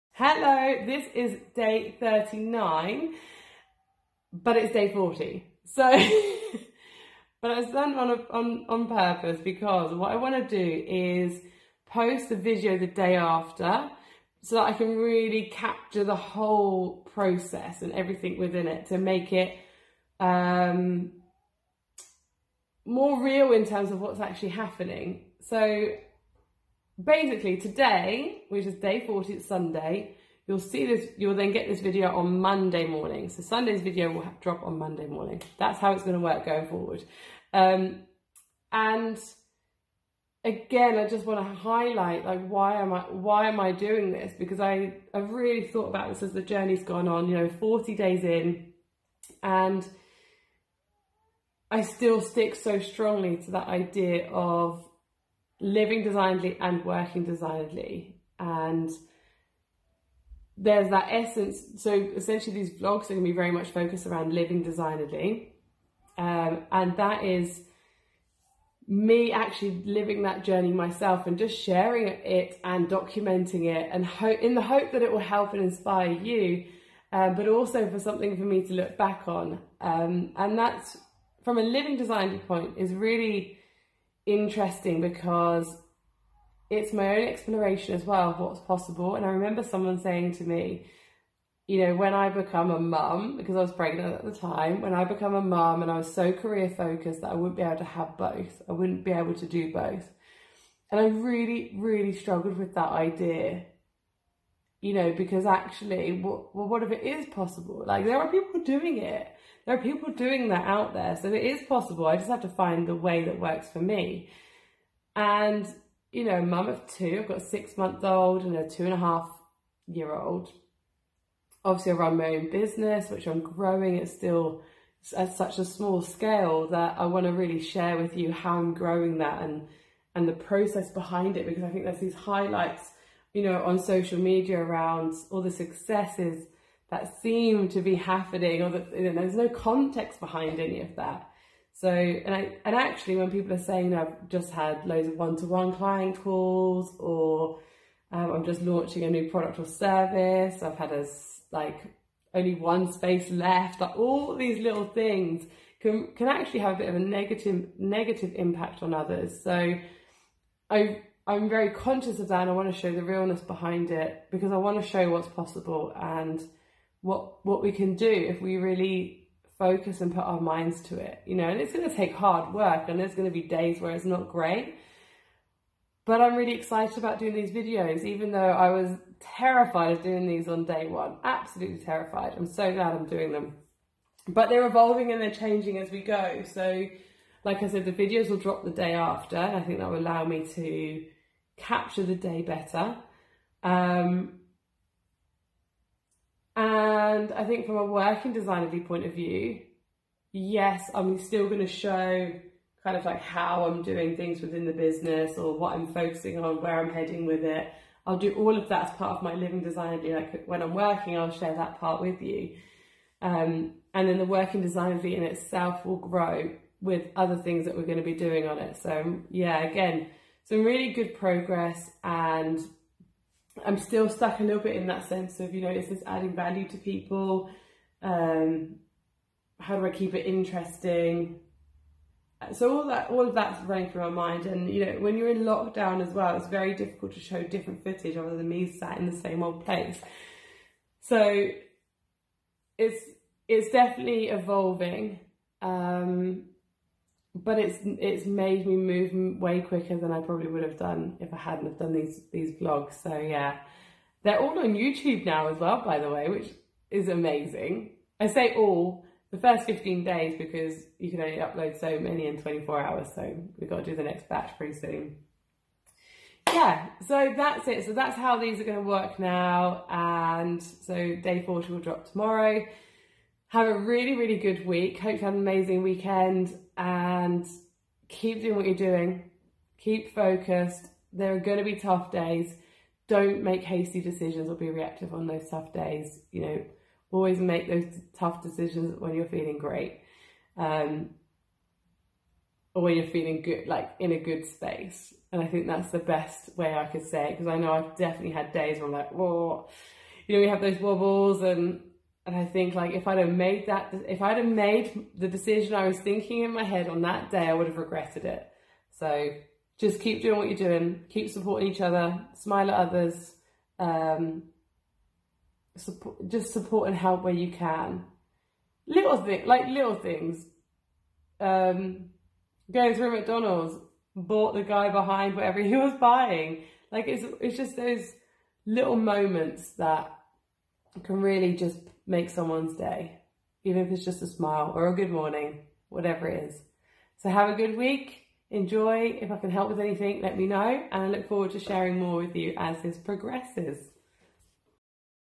MY EXPLORATIONS OF WHAT'S POSSIBLE | LDV daily 39
Hey!
Day 39 is here, the thirty ninth  video of a series of vlogs called "LDV daily"  documenting my journey of both Living and Working Designedly.

Join me as I explore ways to live more designedly, living intentionally, purposely and deliberately, whilst also sharing insight into the growth of my business and brand.
(LDV - a Living Designedly Vision)

Hugely appreciate you taking the time to watch this video and it would be great to hear from you, so please do say 'Hi'.  I hope you stick around for the ride and share your insights with others along the way.